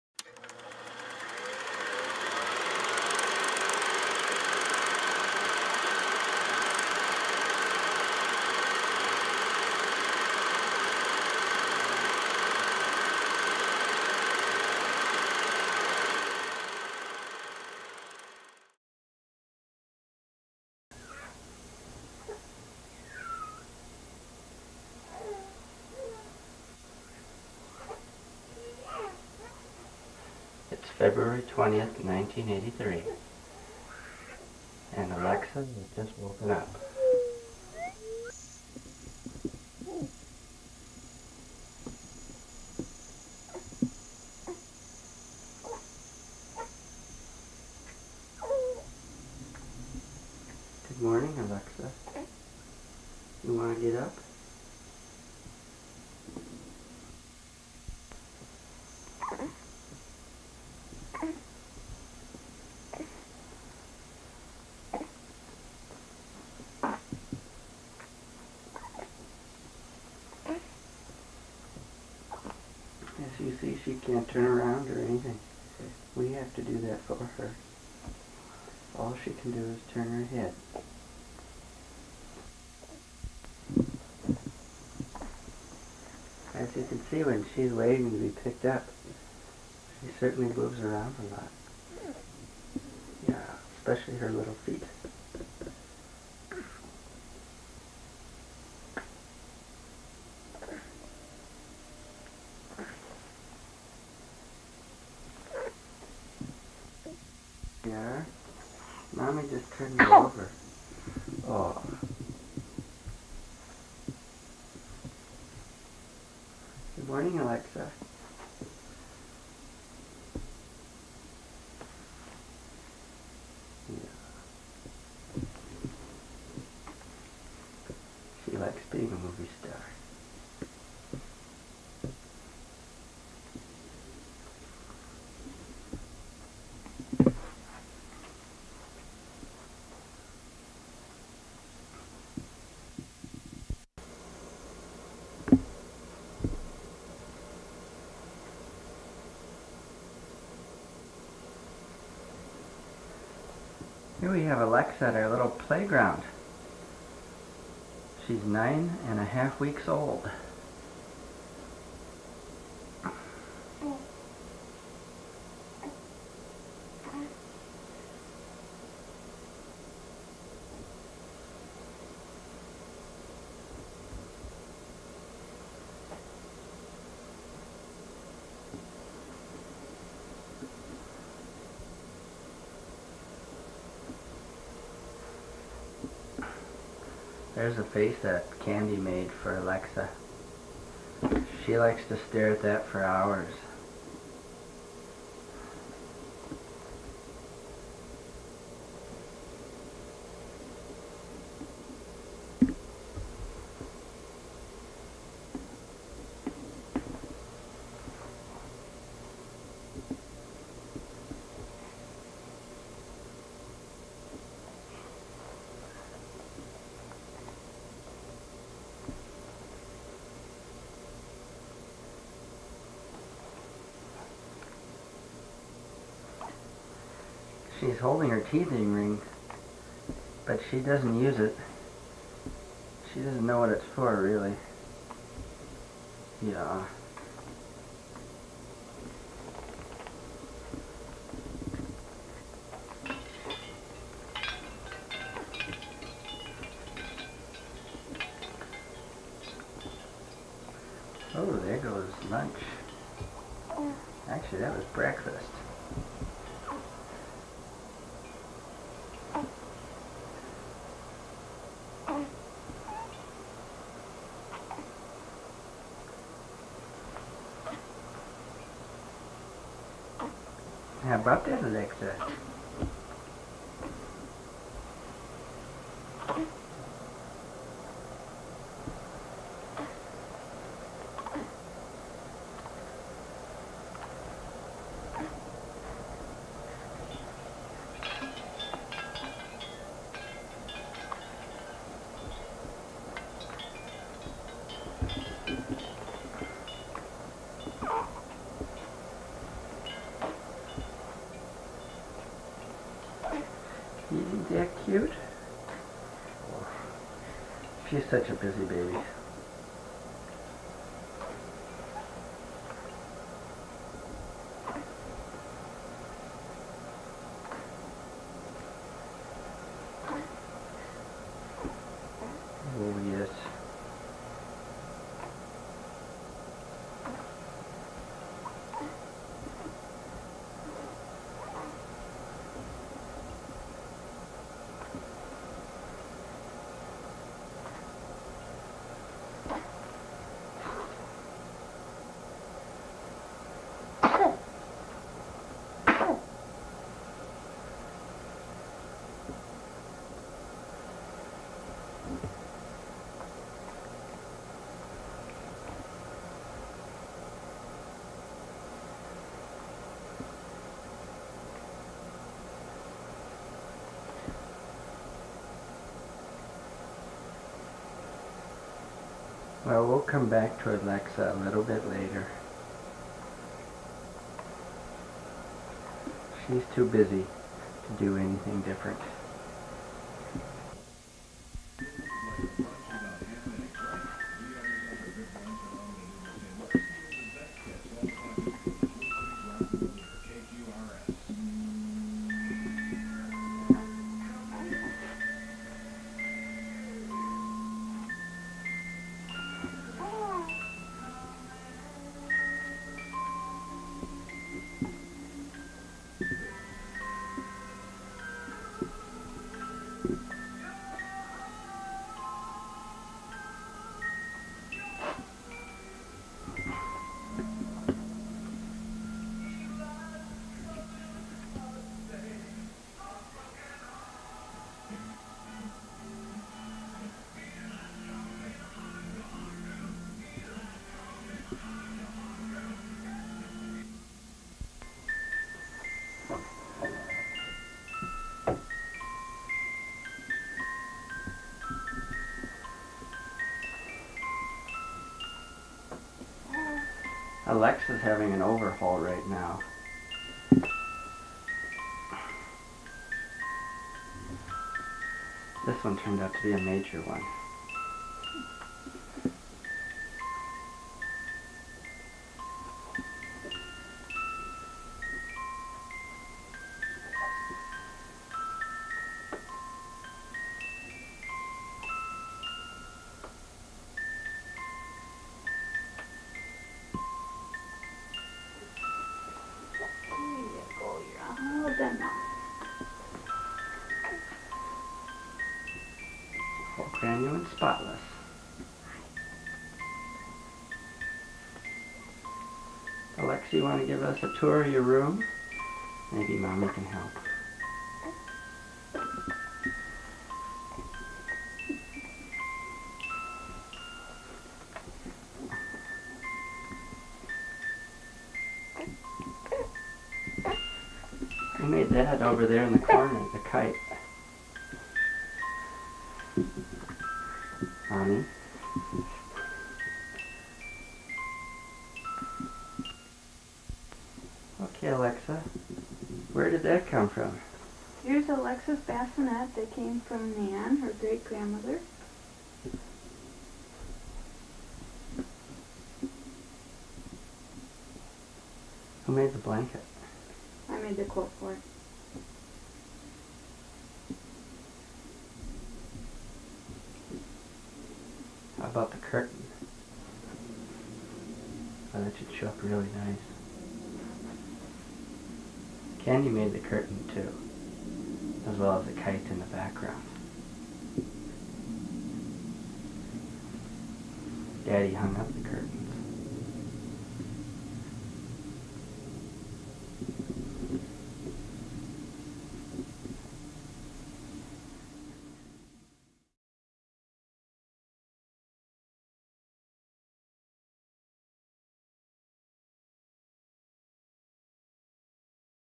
Baby Alexa at home Feb 20, 1983
Here is a short video of Baby Alexa enjoying some quiet time in her room with her parents.  Feb. 20, 1983